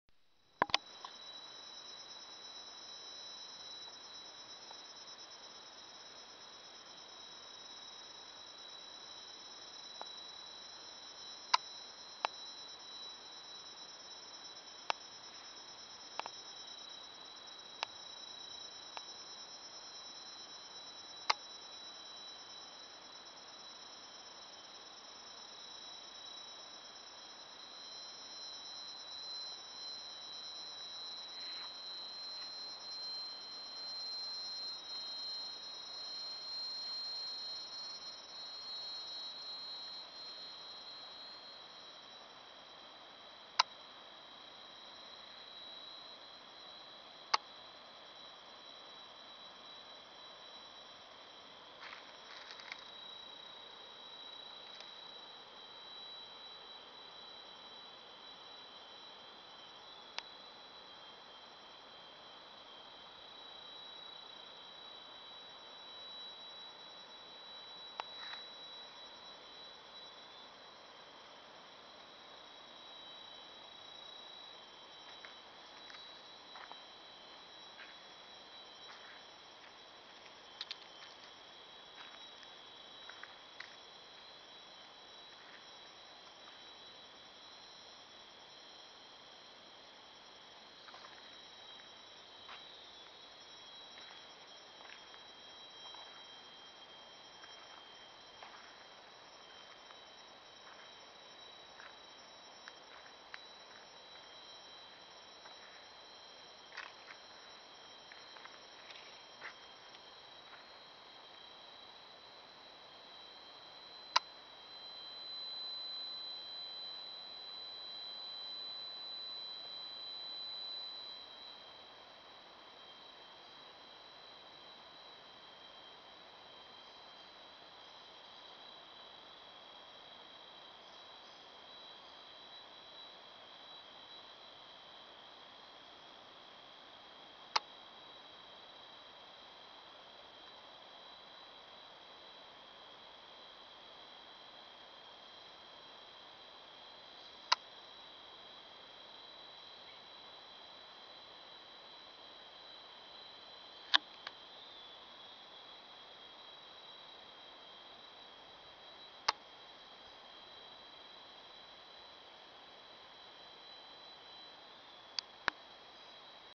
The mountain paths to Mariamman Temple, Kuching, Sarawak.
Fascinating & primitive jungle paths ideal for country side trekking, hash run safari and melting yourselves in the real natural Kuching !. enjoy a natural orchestra from the jungle insects.